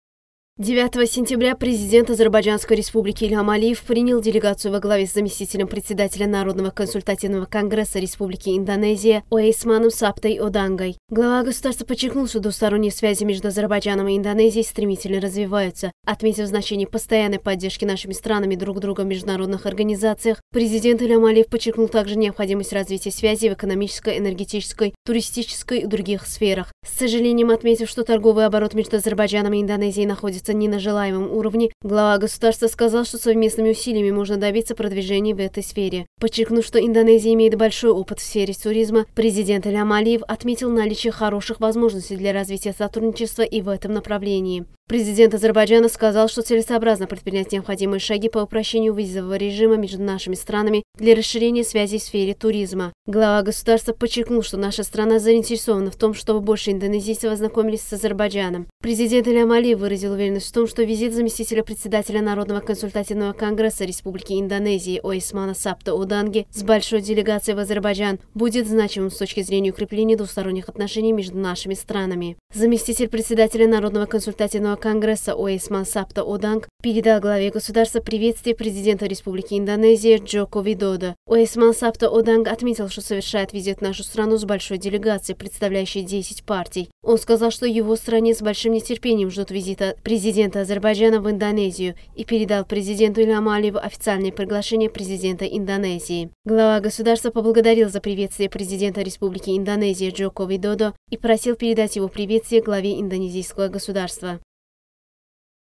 Президент Ильхам Алиев принял Оесмана Сапты Оданги
Президент Ильхам Алиев принял делегацию во главе с заместителем председателя Народного консультативного конгресса Индонезии